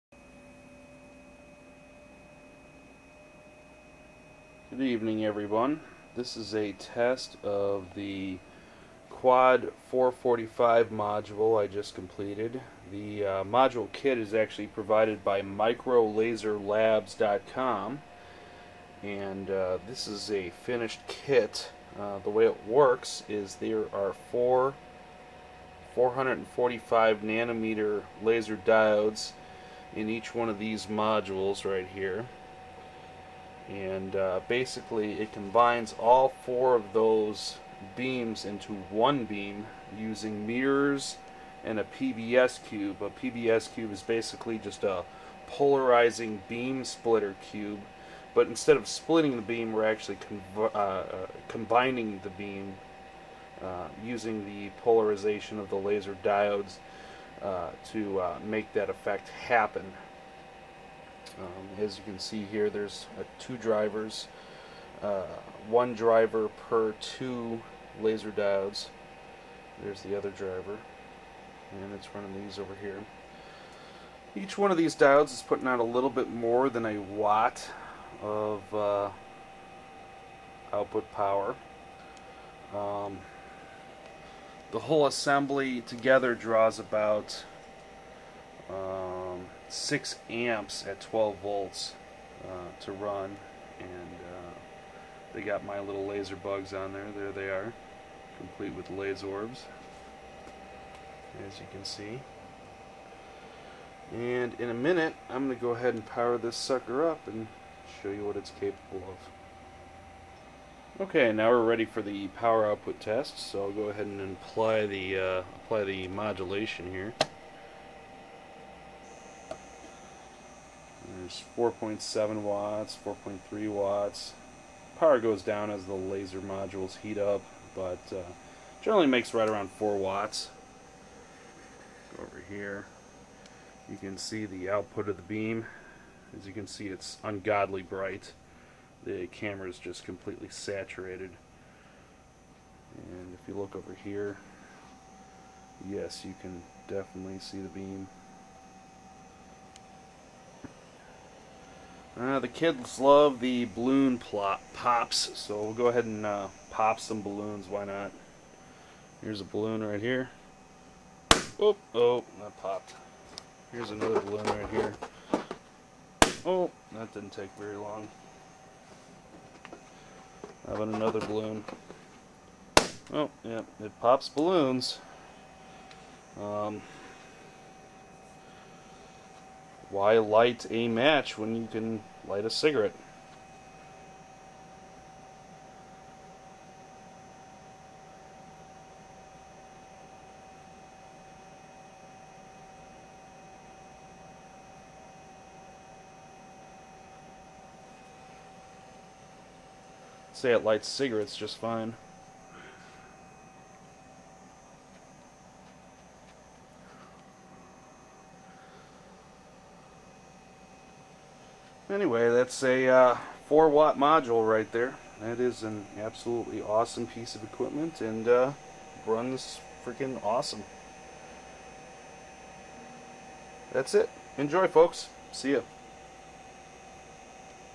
4 Watt 445nm Quad Laser Module Test
This is a 4 watt 445nm quad laser module I assembled form parts you can get

This module has 4 445nm laser diodes running a little over 1 watt a piece and draws 6 amps @ 12VDC to drive it at full output power. The laser also has analog modulation input. This module will be used for light show applications at a large club.